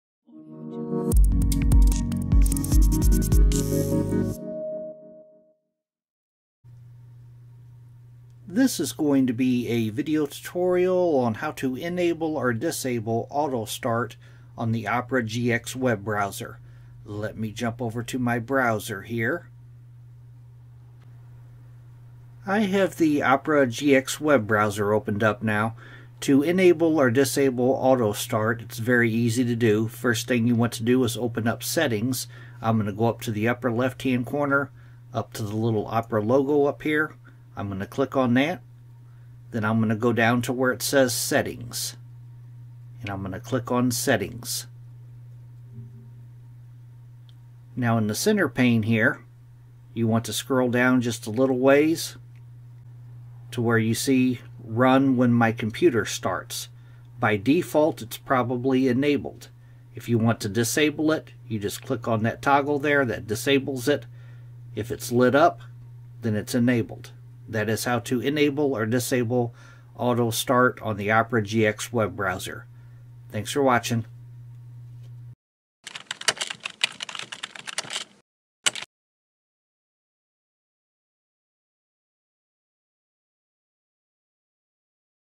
How To Enable Or Disable Automatic Startup On The Opera GX Web Browser | PC Tutorial
How To Enable Or Disable Auto Start On The Opera GX Web Browser | PC Tutorial

In this tutorial, you'll learn how to enable or disable the Auto Start feature on the Opera GX web browser using a PC. Auto Start allows Opera GX to launch automatically when you start your computer, but this may not be ideal for everyone. We'll show you step-by-step how to control this setting, so you can choose whether or not Opera GX starts automatically. Perfect for customizing your browser's behavior to match your needs!

Simple Steps:
1. Open the Opera GX Web Browser.
2. Click on the "Red O Logo" in the upper left corner and choose "Settings".
3. Scroll down the center pane to the "Run When My Computer Starts" area and toggle it on or off.

Chapters:
0:00 How To Turn Off Or On Automatic Startup On The Opera GX Browser
0:20 Open The Opera Gx Browser
0:30 Click On The Red O Logo In The Upper Left Corner
0:40 Select Settings
0:50 Toggle On or off Run When My Computer Starts